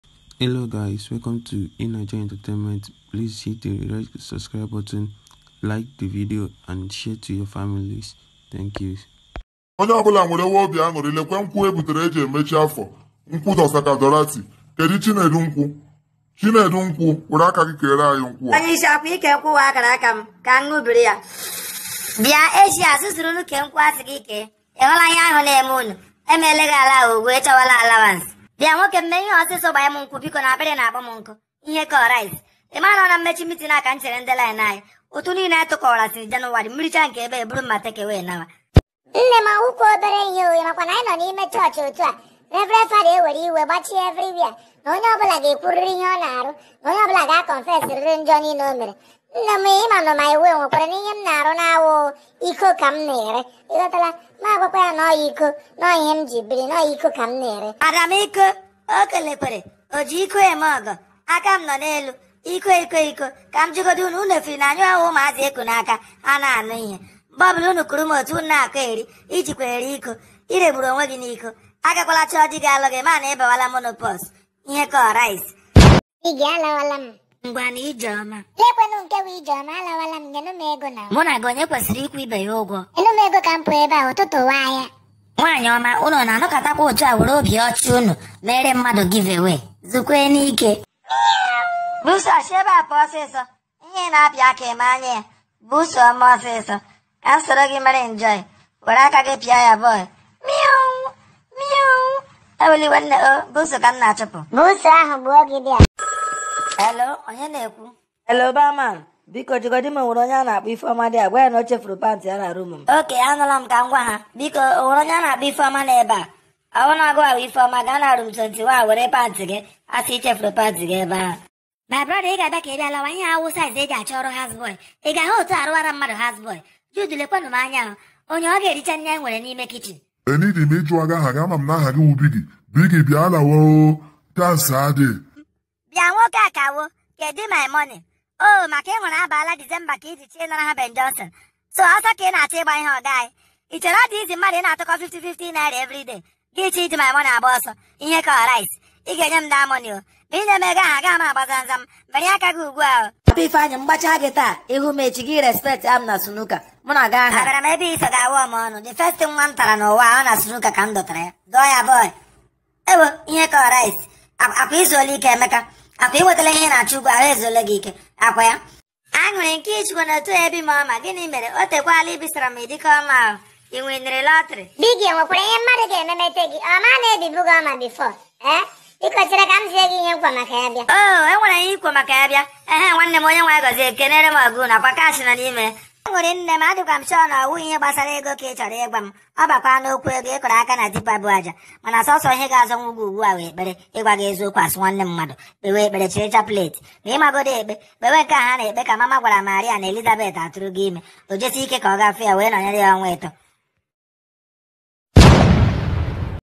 Best funny comedy compilation of insidelife411 2020(laugh and get tired)
Watch funny comedy compilation of insidelife411, in this episode you will see papa Ifanyi. Thank you.